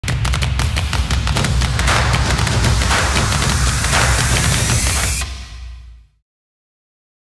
Channel Intro
This channel is mainly just PS5 trophy uploads for anyone who may find them helpful or is looking for a guide.

No subscription required
No live commentary
No ads